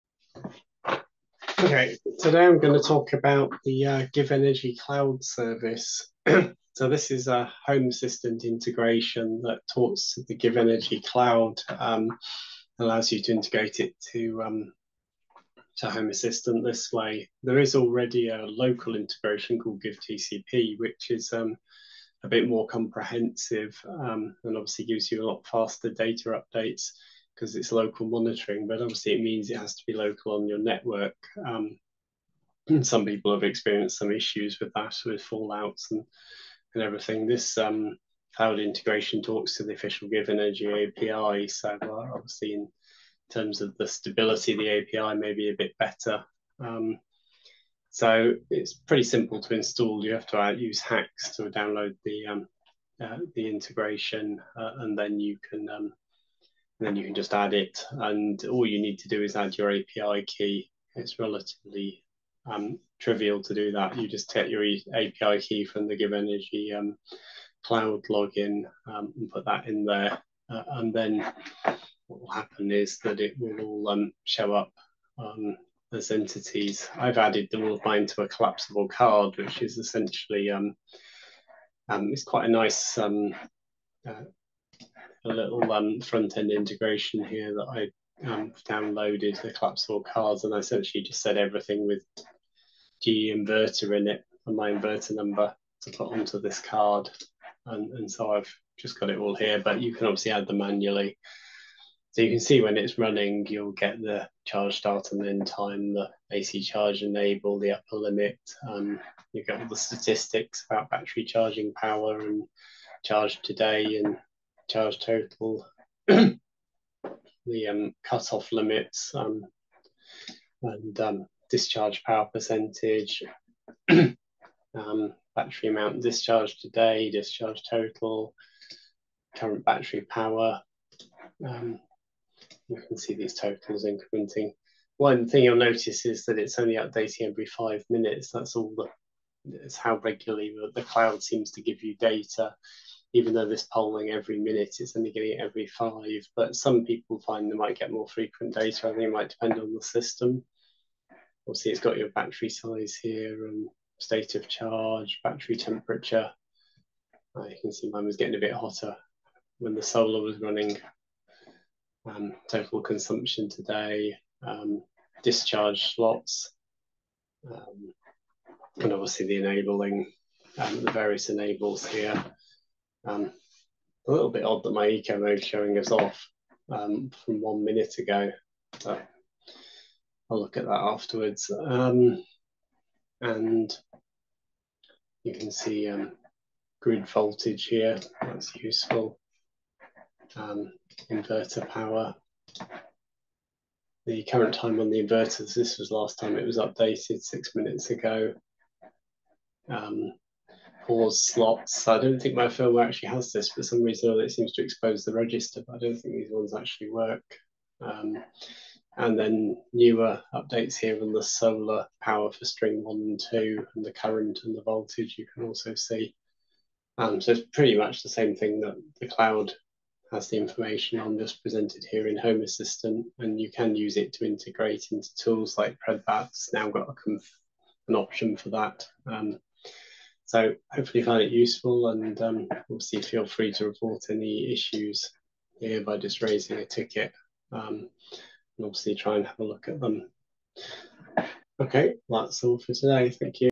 HomeAssistant - Givenergy Cloud Integration (updated)
GivEnergy Cloud integration for Home Assistant allows Givenergy inverters to be monitored and controlled via the cloud.